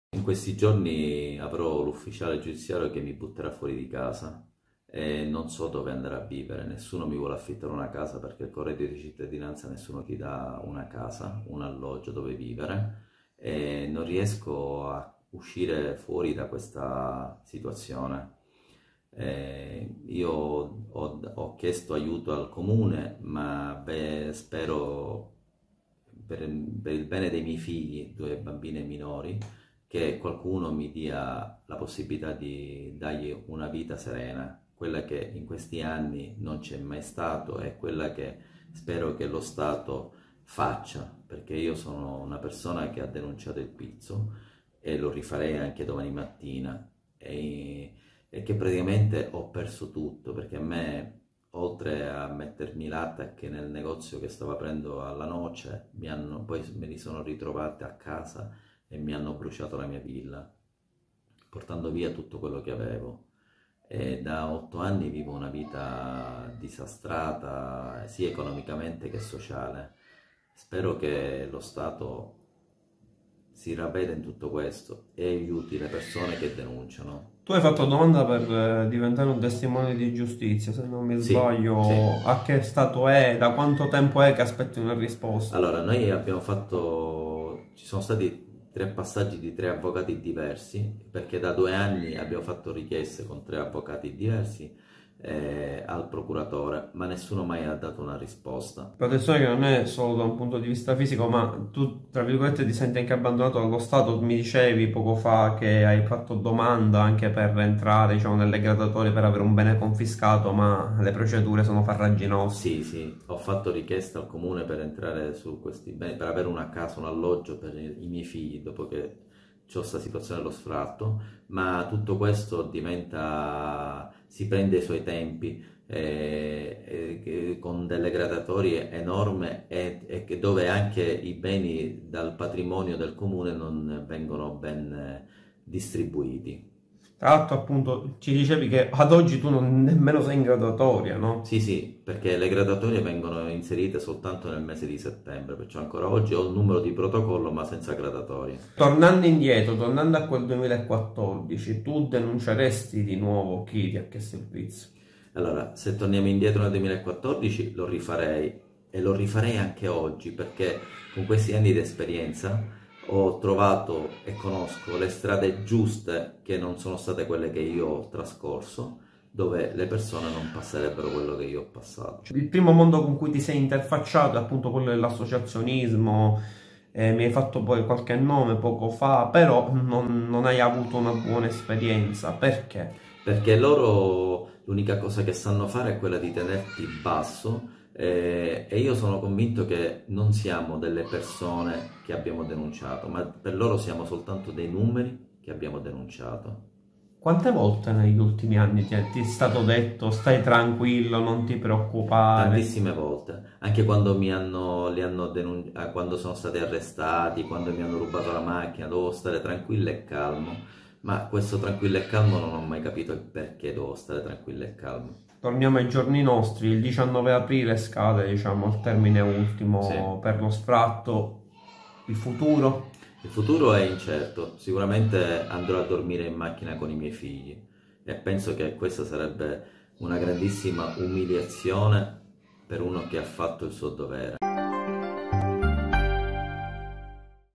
"Ho denunciato il pizzo, ora rischio sfratto"
Giuseppe Balsamo, imprenditore palermitano che ha denunciato il pizzo, si trova oggi sull'orlo di uno sfratto. Il racconto della sua storia.